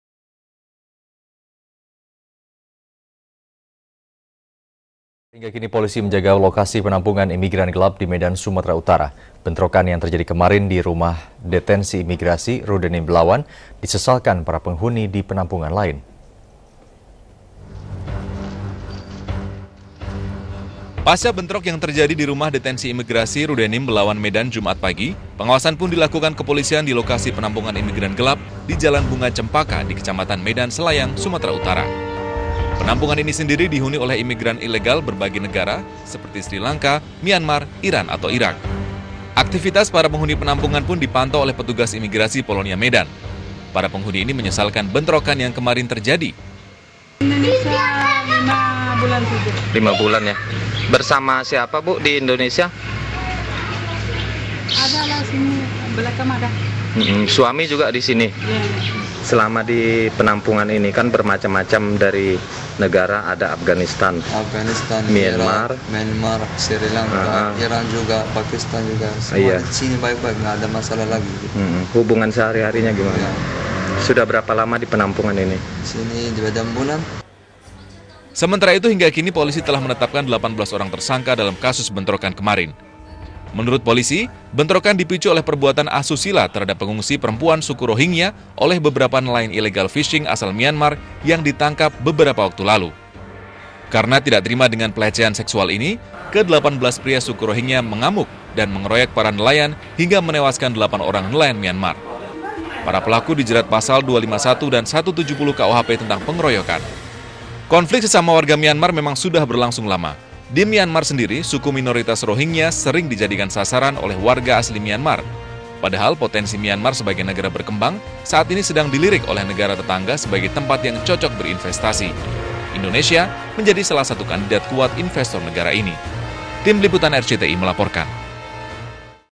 Pasca Bentrok, Rumah Pengungsian di Awasi Polisi
Pasca bentrok antara pengungsi Rohingya dengan nelayan Myanmar, 18 orang ditetapkan sebagai tersangka.